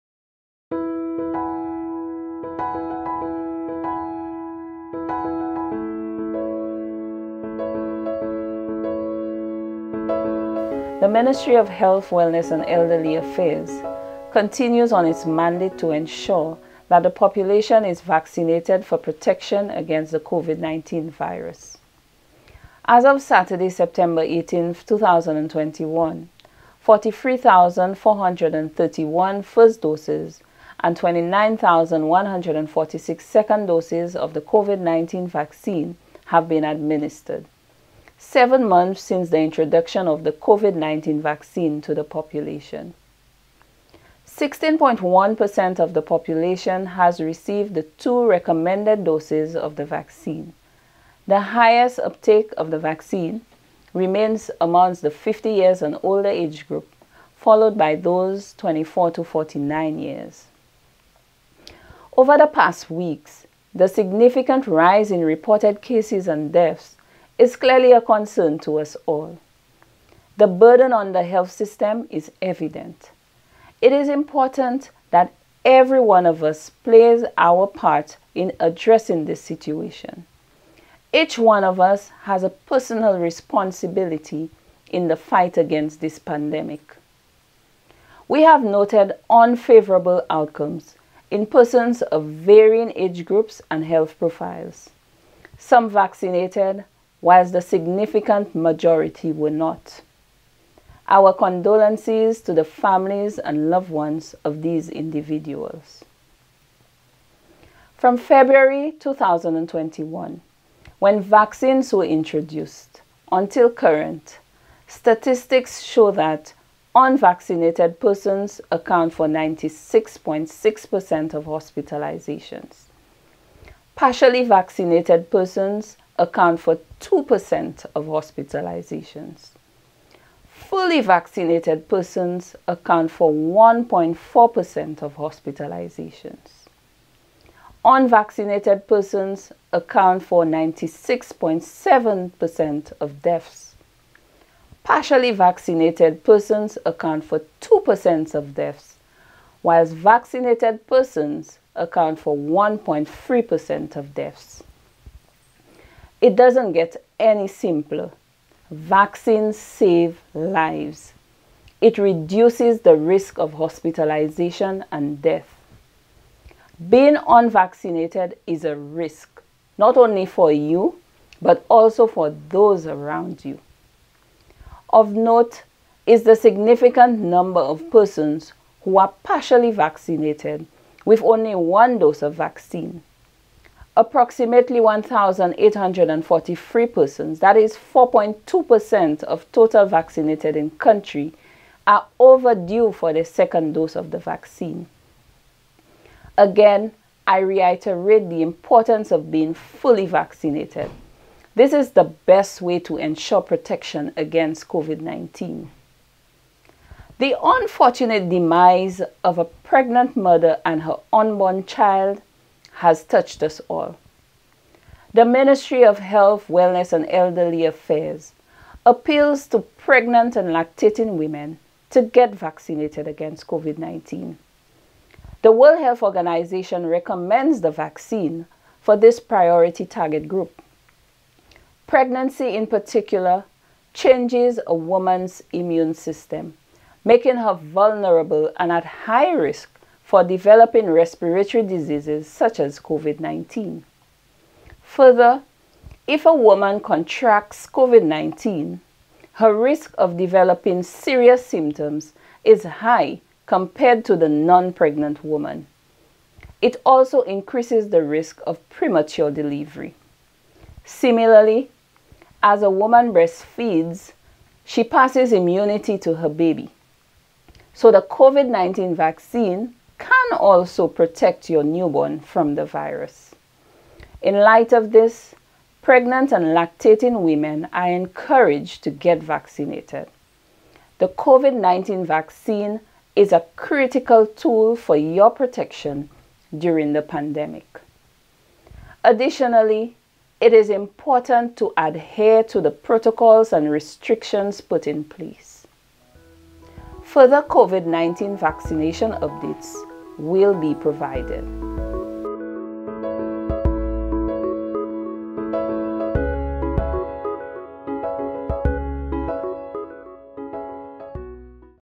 Ministry of Health stresses the importance of vaccinations
The Ministry of Health has noted unfavourable outcome in persons of varying age groups and health profiles. Some vaccinated, whilst the significant majority were not.
From the February 2021, when vaccines were introduced, until current, statistics show that: Unvaccinated persons account for 96.6% of hospitalizations; Partially vaccinated persons account for 2% of hospitalizations; Fully vaccinated persons account for 1.4% of hospitalizations; Unvaccinated persons account for 96.7% of deaths; Partially vaccinated persons account for 2% of deaths; Vaccinated persons account for 1.3% of deaths.
- - Address by Tecla Jn Baptiste, National Immunization Manager